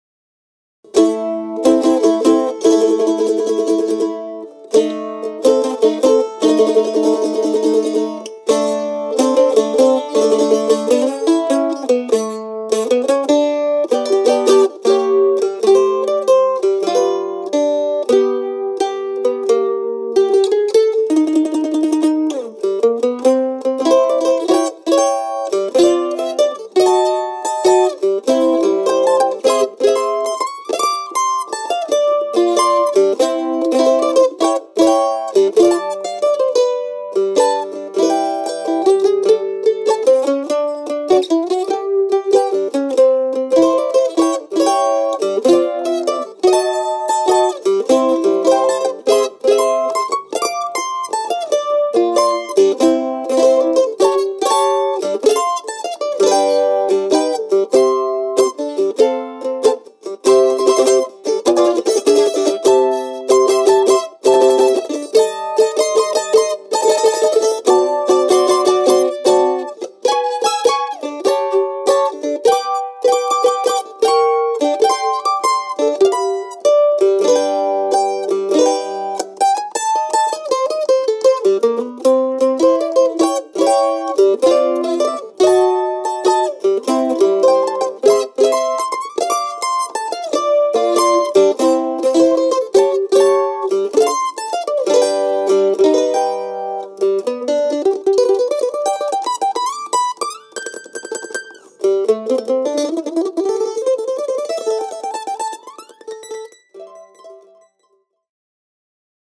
Weber Fine Acoustic Instruments: Fern F Mandolin Video
In this video we show off our most traditional and prestigious mandolin, the Fern is a perfect synthesis of heart-stopping tone and heirloom looks. Made with the finest of our select tone woods, our Fern brings unparalleled chop, sustain, and volume to any setting, whether it be a jam session or professional recording.